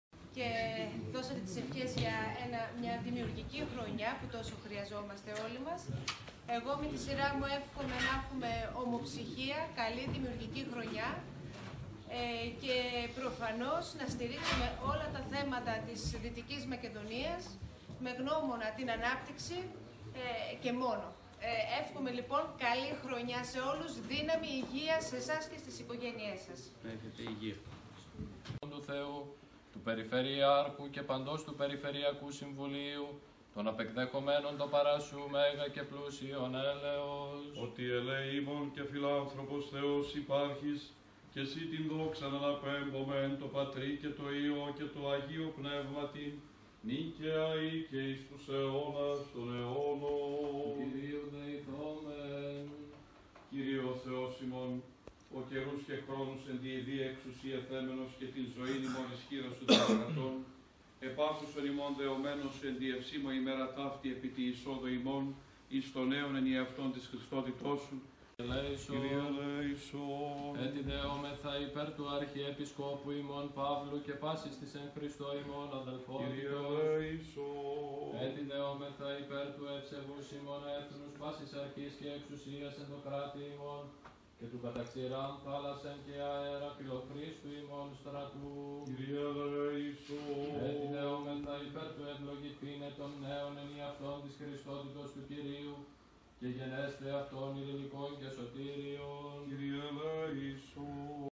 kozan.gr: Έκοψαν πίτα στο Περιφερειακό Συμβούλιο Δ. Μακεδονία για τη νέα χρονιά (2023)
Έκοψαν πίτα στο Περιφερειακό Συμβούλιο Δ. Μακεδονίας, στην πρώτη συνεδρίαση, για τη νέα χρονιά (2023).